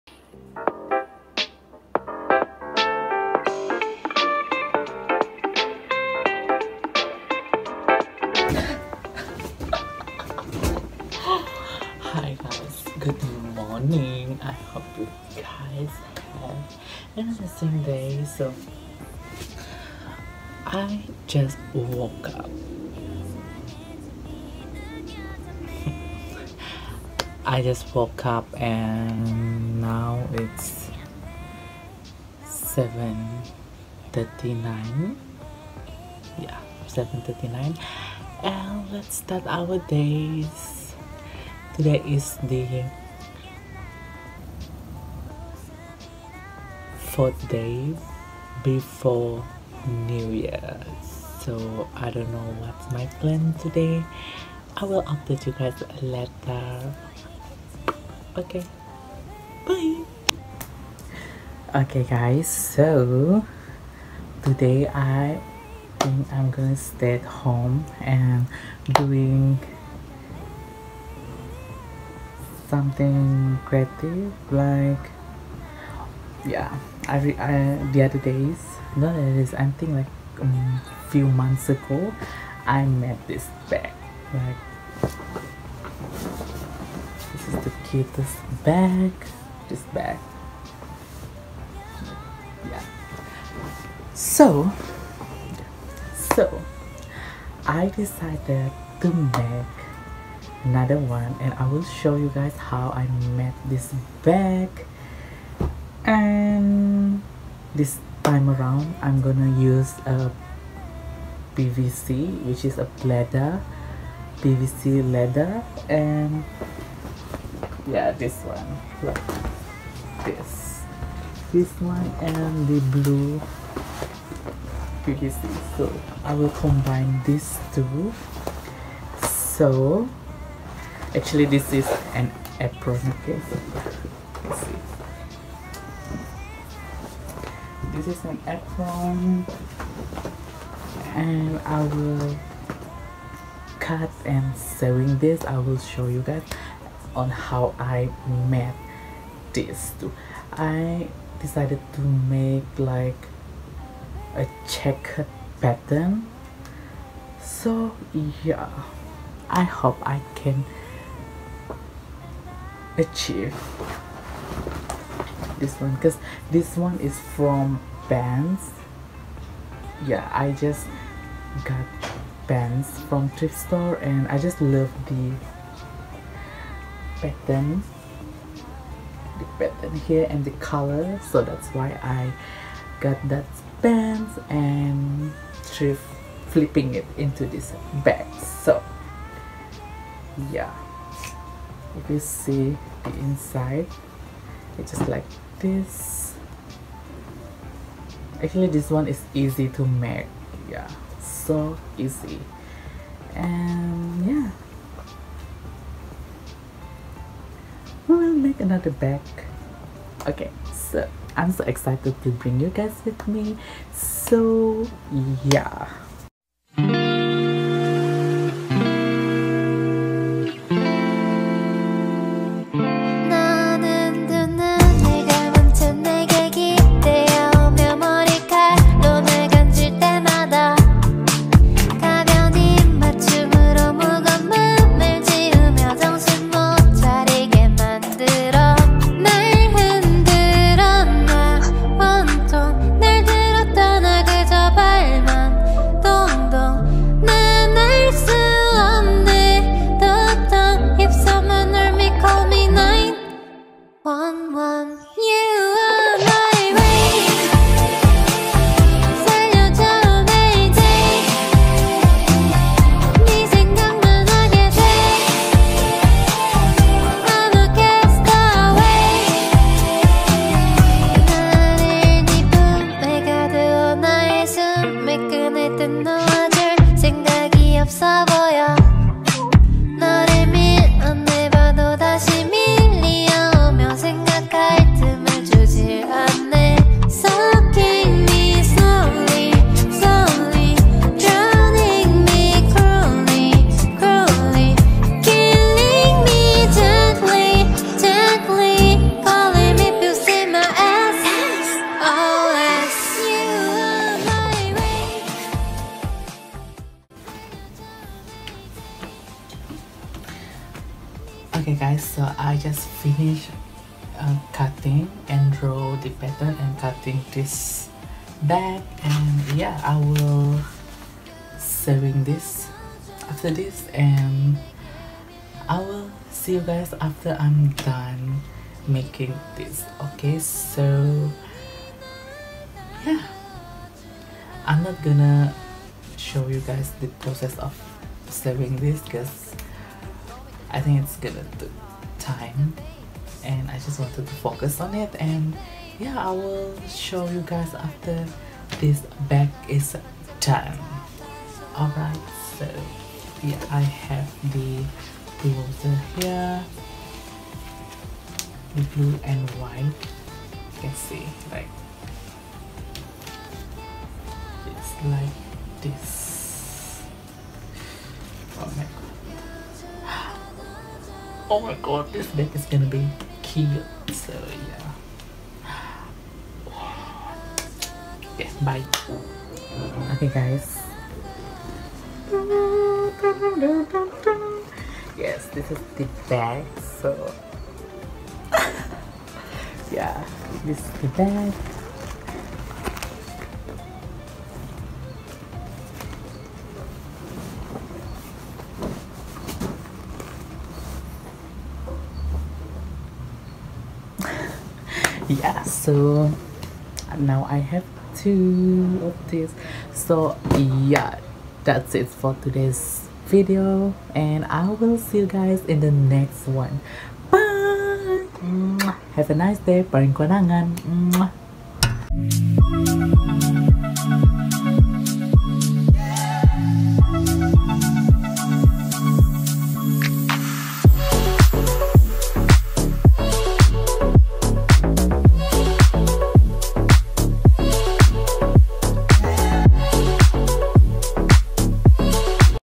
I MADE CHECKED BAG
Hi guys it’s me Jun here !

Lets make new checked bag hahaha I know it so out trend but still I really want to make one for me!!! have a great day I'll see ya in the next one :)

Paringkuanangan